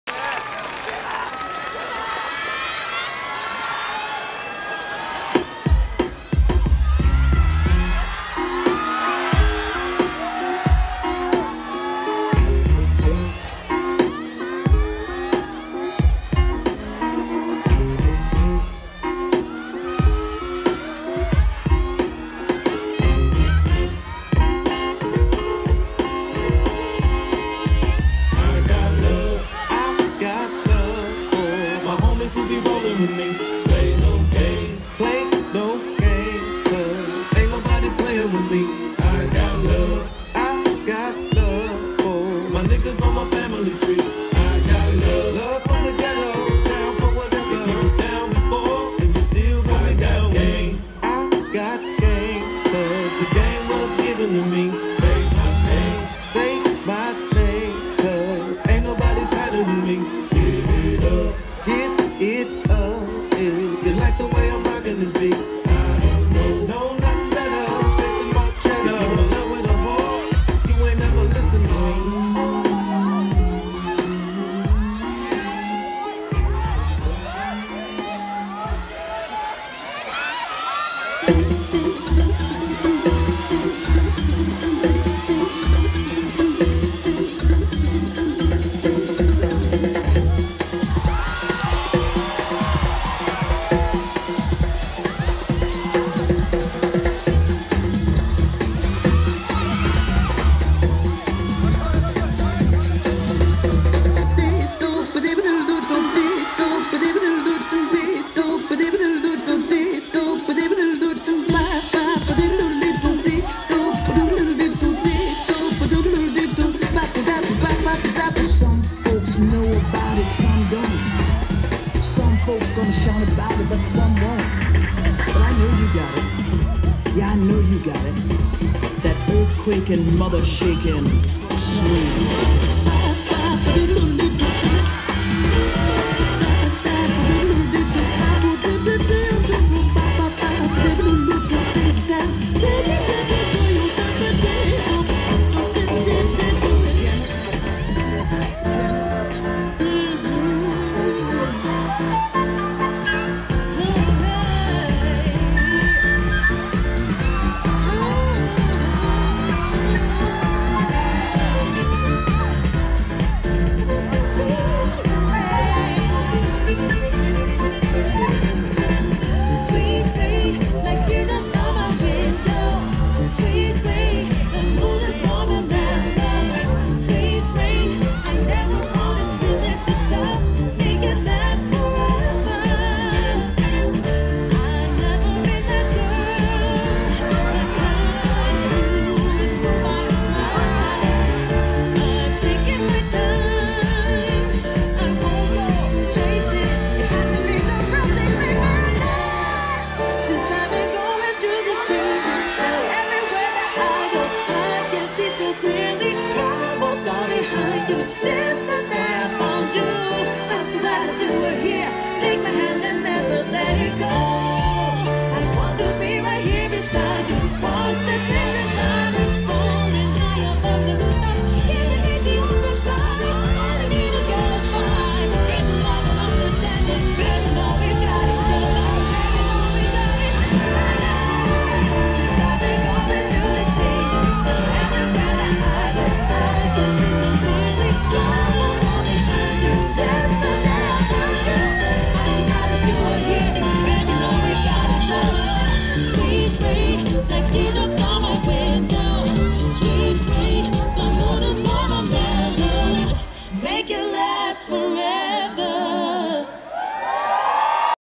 【punking】dance crew es -est 2017 メインステージ公演
慶應義塾大学公認サークルdance crew esによるパフォーマンス

詳しい情報はこちらで随時更新中です！！